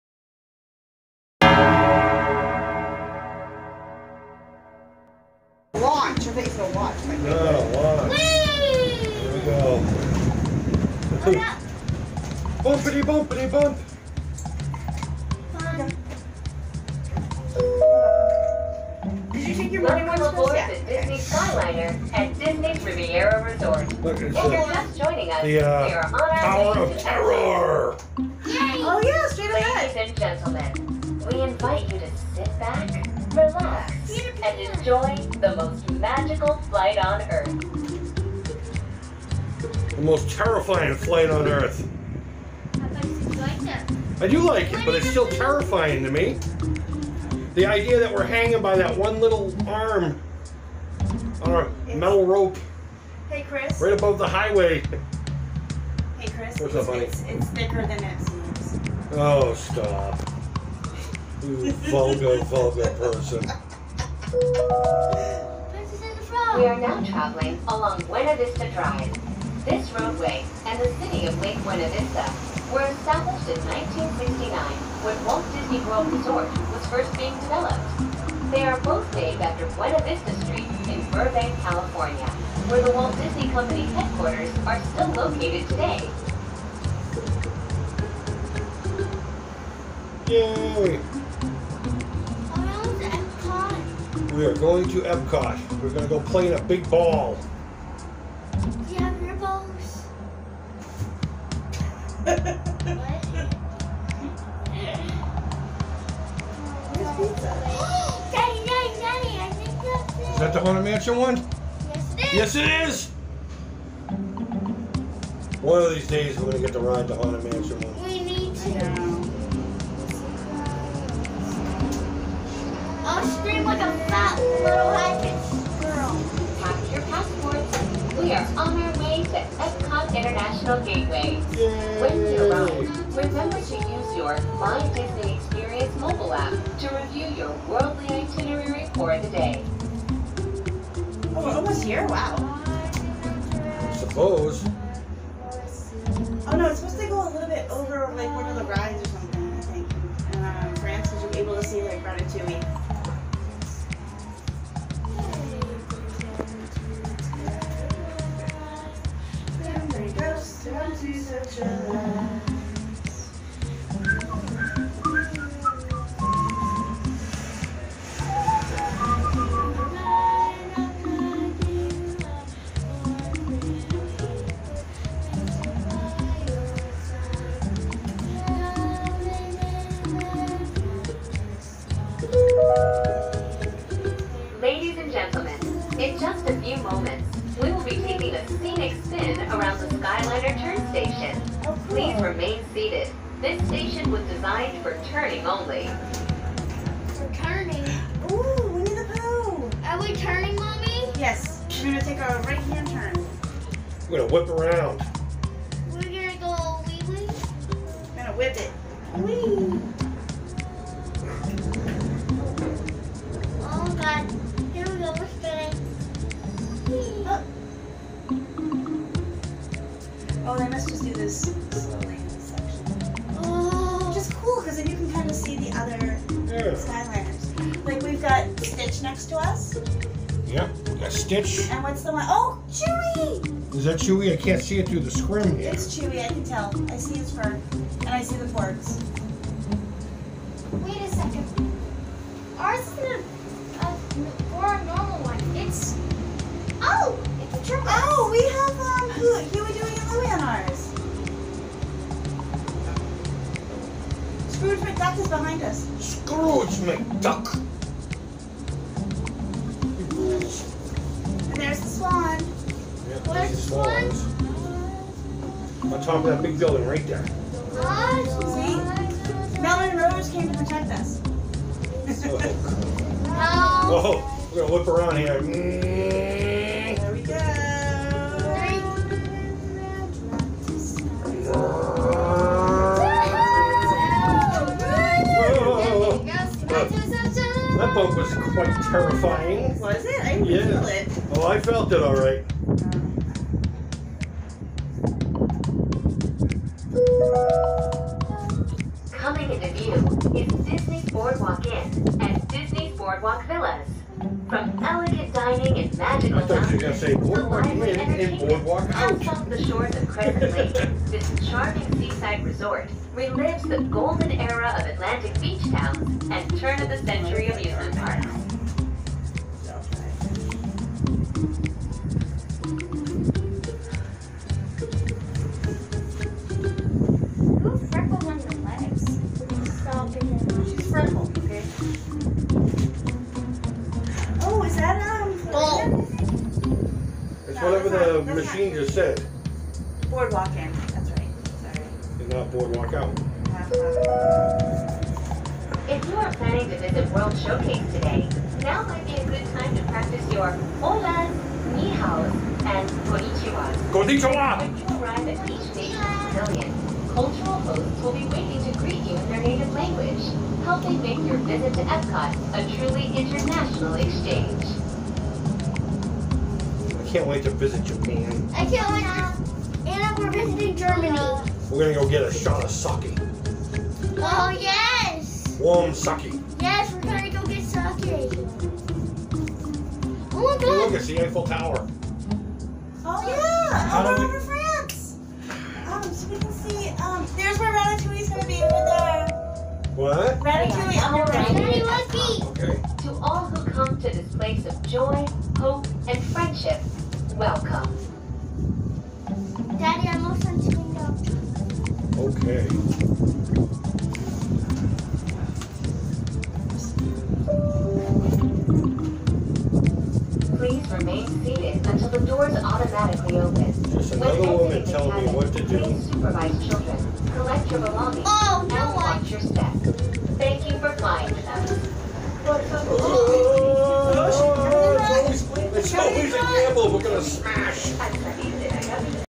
Ride to Epcot on the Disney Skyliner 2021
Hey Creepers,

Come with us as we cruse the skys in the Disney Skyliner. No narrative, just a relaxing rise.